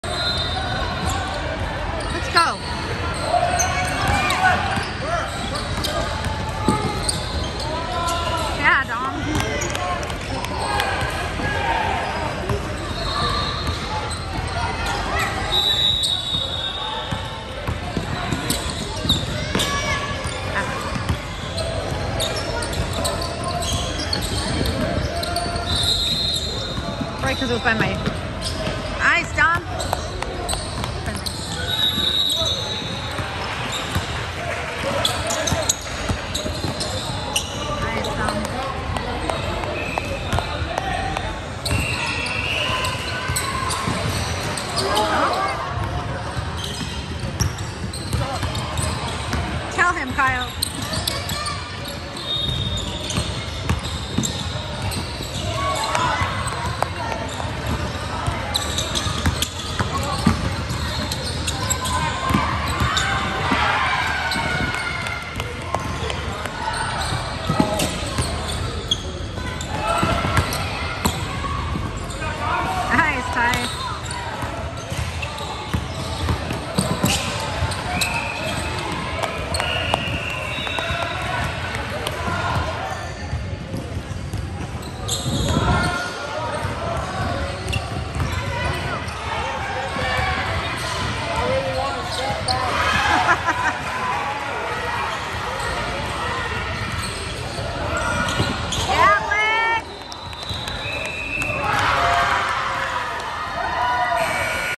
Dominic Marrujo #13 (2022) Colorado Empire vs Team Hydras JPSports: Varsity 3v3 Basketball 2021
JPSports: Summer Varsity 3v3 Basketball Tournament 2021